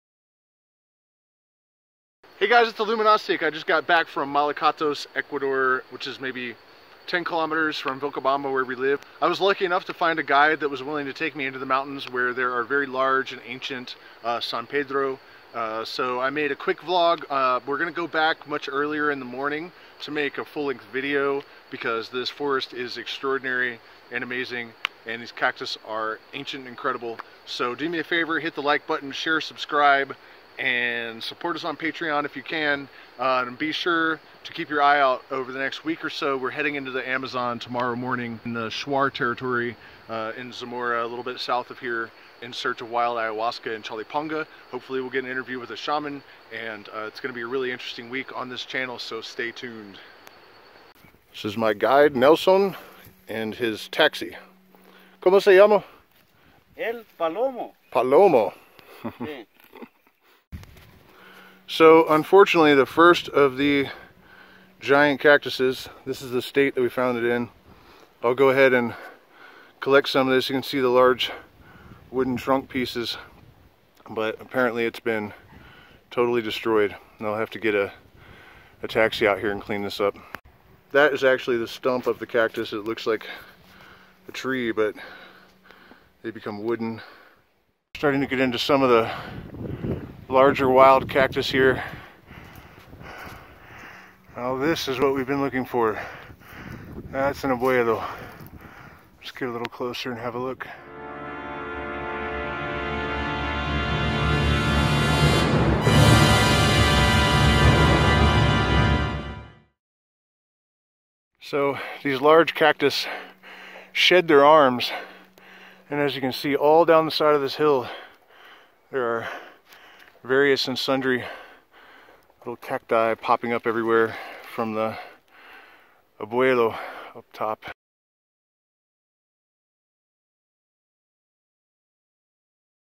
The Ancient San Pedro Forest of Malacatos, Ecuador
Situated amongst the Andean peaks in southern Ecuador, Malacatos is named for the Malacatus tribe that ruled over a half dozen smaller tribes in the area. The hills surrounding the town are peppered with thousands of sacred San Pedro Cacti, some of them incredibly old, and massive.
One of our farm-hands at the my late mothers finca brought me into these extraordinary cactus forests a few months back, and I made this vlog of my recent trip.
Unfortunately, the largest of these Abuelo's was cut to the ground, presumably by the local ranchers. Why, I can't imagine. You will see the wreckage in this vid. You have been warned. You will also see quite healthy cacti as well.
I will return to this place in a few weeks to make a full length vid, which will include harvesting with traditional tobacco blessing, and a bit more info about the San Pedro Cactus.  I apologize for the shaky camera. The full length vid will be smooth. Aho!